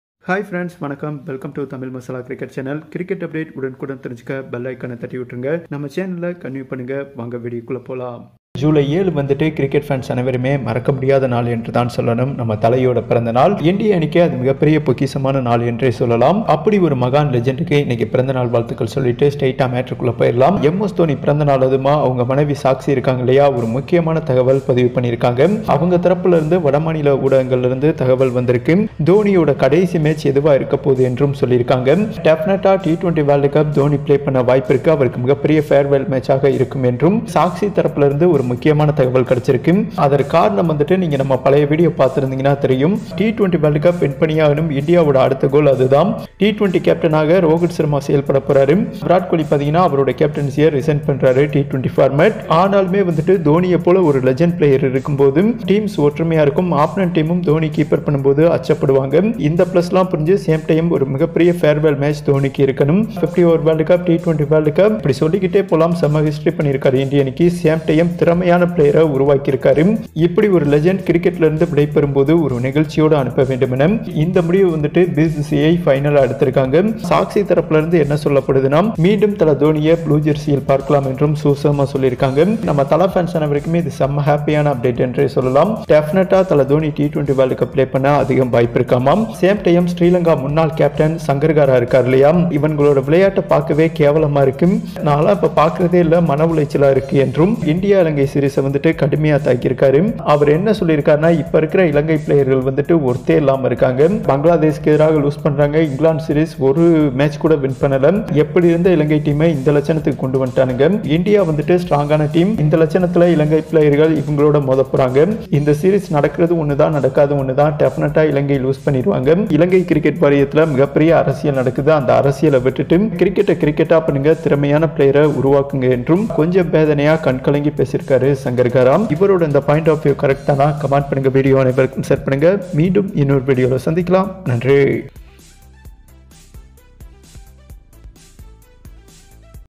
மீண்டும் புளூ ஜெர்சில, தோனியை பார்க்கபோறீங்க! ஷாக்சி முக்கிய தகவல் | Ind vs Sl Series
Happy Birthday MS Dhoni: Five times 'captain cool' MSD shocked fans with bold decisions that won India matches

On his 40th birthday, here is a look back at five moments from MS Dhoni's career that became a testament to his captaincy:

Today Cricket News | Cricket Tamil News `
 கிரிக்கெட் செய்தி | IPL 2021News | Cricket Latest News